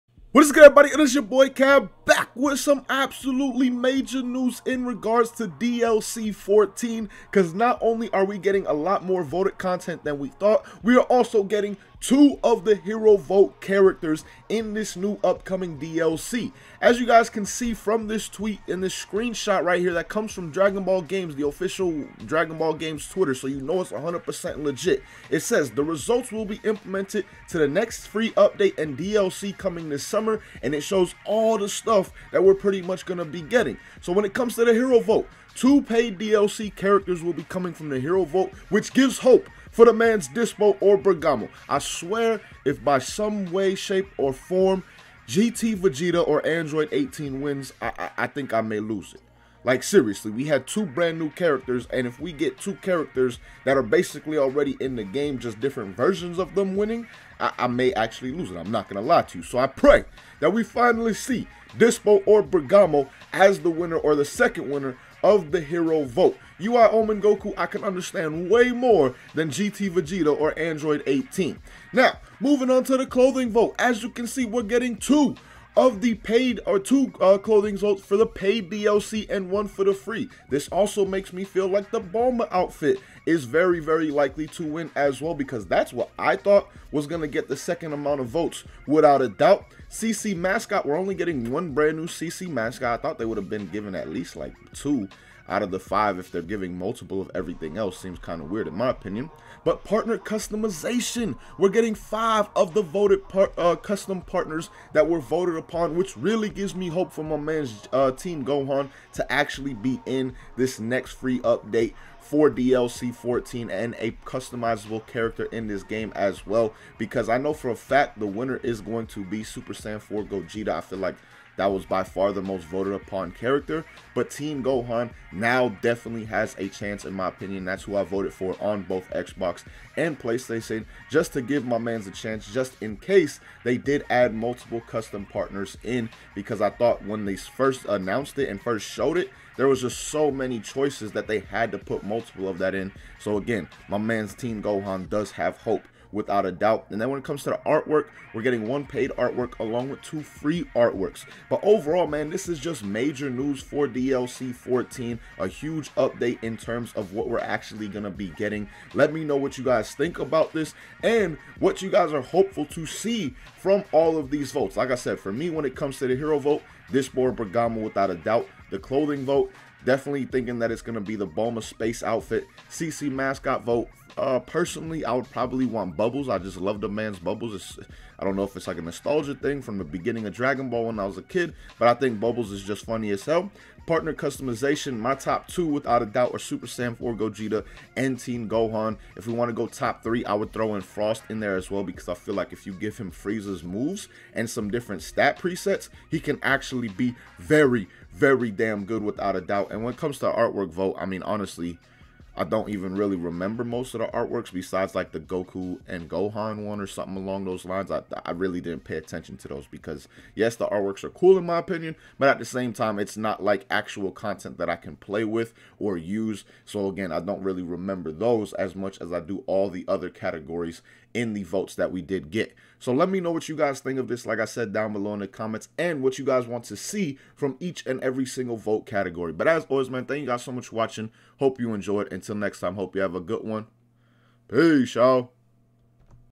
Xenoverse 2 Huge DLC 14 Update! Free & Paid Update Info!
As always thank you guys for watching I hope you guys enjoyed this dragon ball xenoverse 2 video going over the some major news that we got in regards to dlc 14 as well as all the different conton city votes! It looks like we will be getting multiple hero vote characters and multiple of pretty much every single kind of vote that was done at the same time. Let me know what you guys think of this down below in the comments and what you wanna see from all these different kinds of votes!